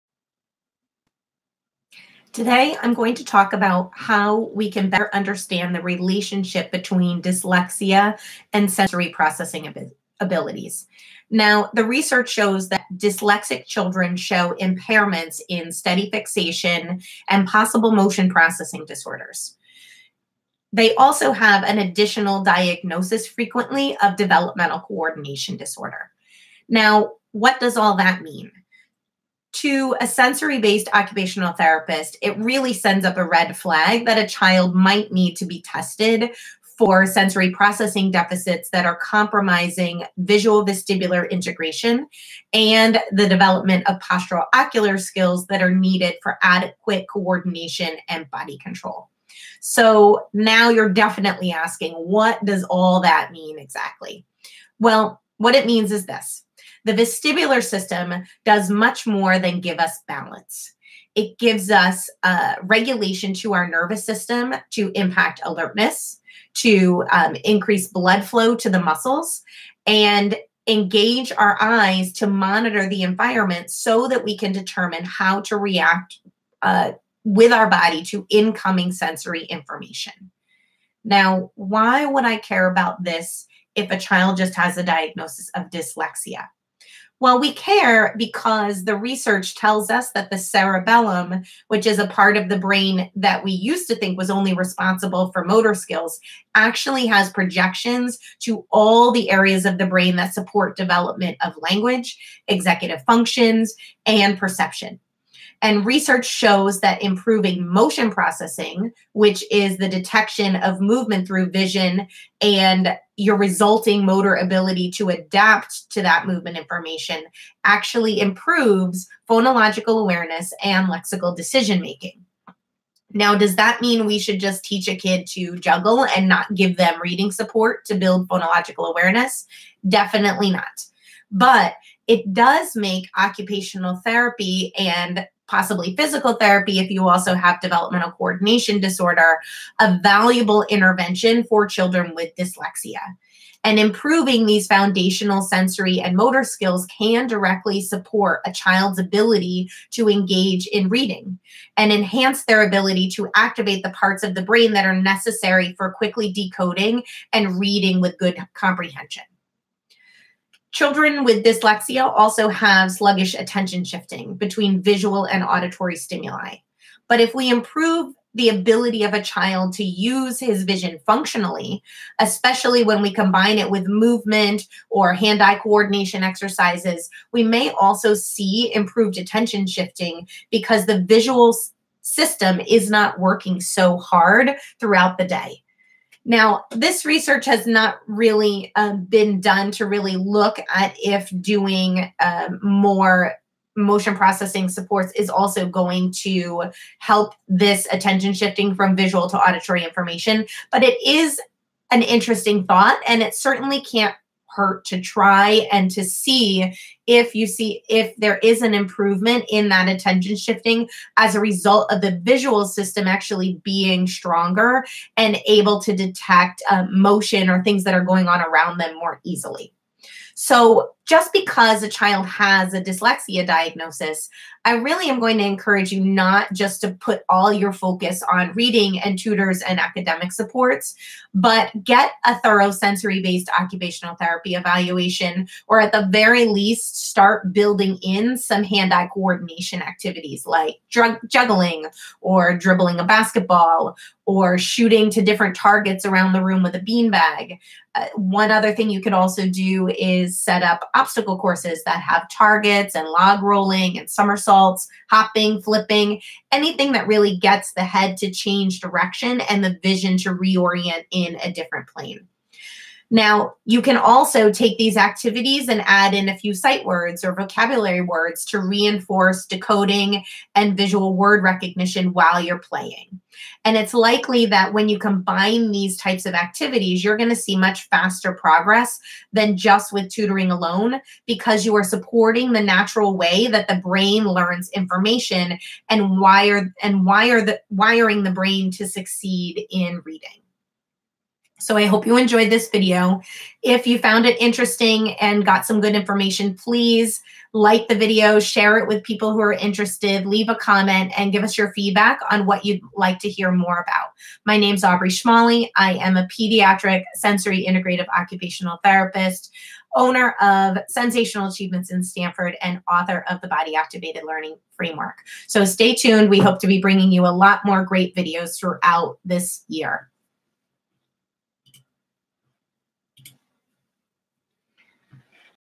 Understanding dyslexia and sensory processing
Children with dyslexia often have motion-processing disorders and may have an overlapping diagnosis of Developmental Coordination Disorder.  It is important to understand how the brain and body work together to support reading skill development in order to make sure that you are engaging a child with dyslexia in activities that are going to support development of visual skills and phonological awareness.  This involves not just direct reading instruction but a sensorimotor approach to increase connections in the brain that increase the brain's ability to draw on sensory information for reading skill development.